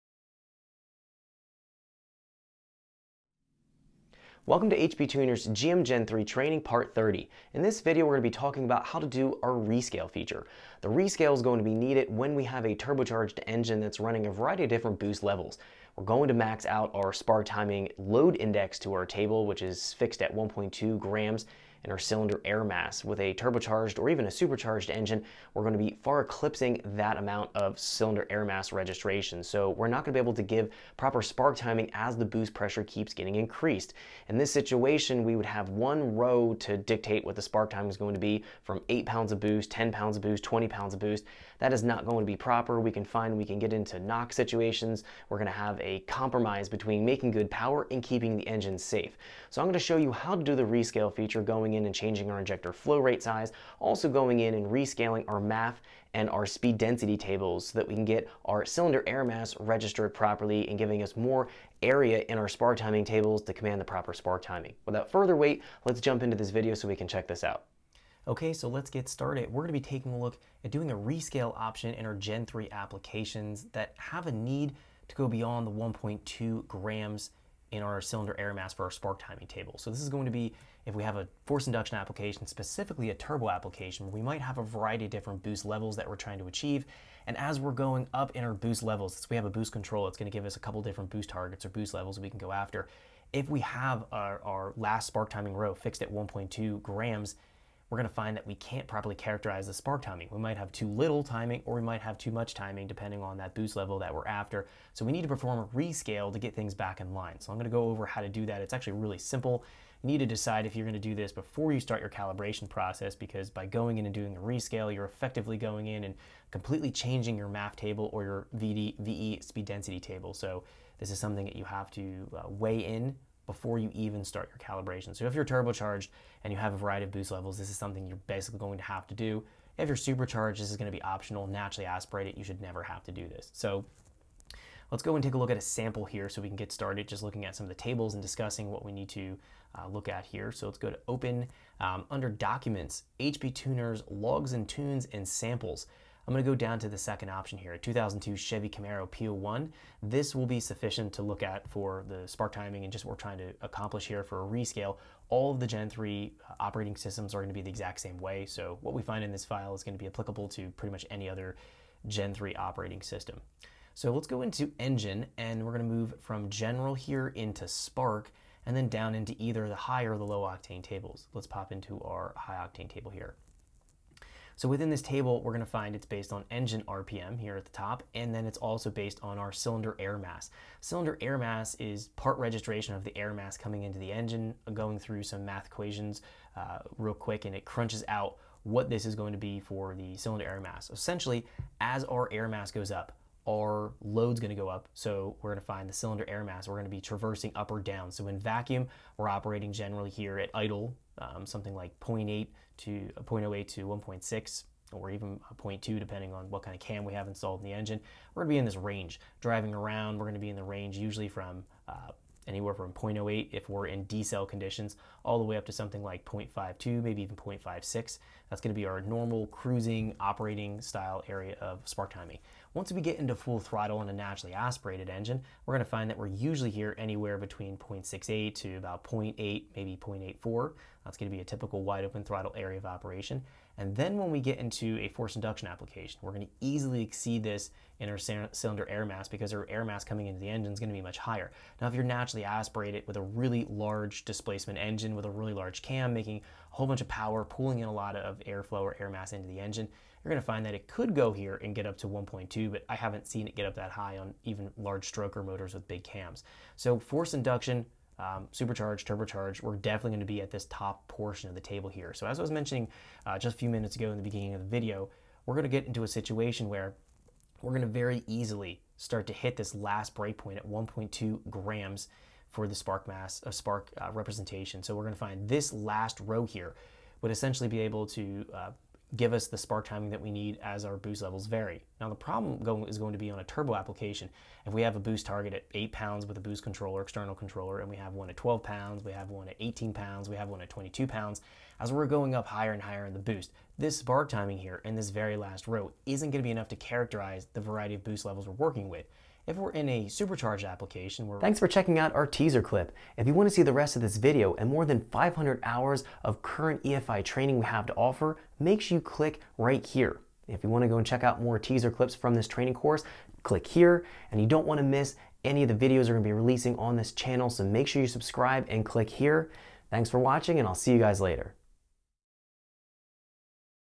HP Tuners GM Gen III Training Part 30: Spark Table Rescaling | Evans Performance Academy
HP Tuners GM Gen III Training Part 30: Spark Table Rescaling walks you through how to perform a rescale in order to get the cylinder airmass registration to give double resolution in the spark timing tables.